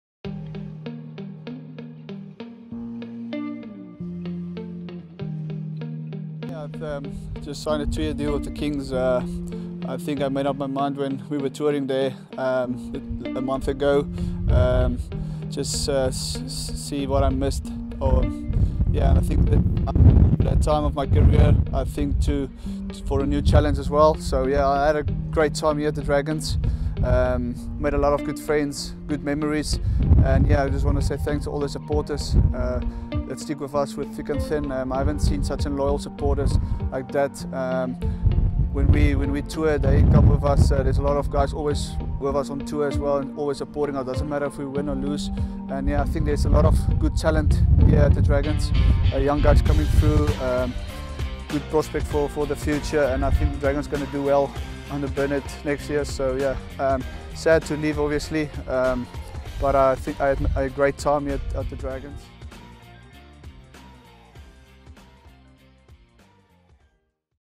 Sarel Pretorius Message to Supporters
Dragons scrum half Sarel Pretorius has decided to return to South Africa to join Southern Kings on a two year deal

Sarel will play his last game for the Dragons at Rodney Parade tomorrow against Toyota Cheetahs.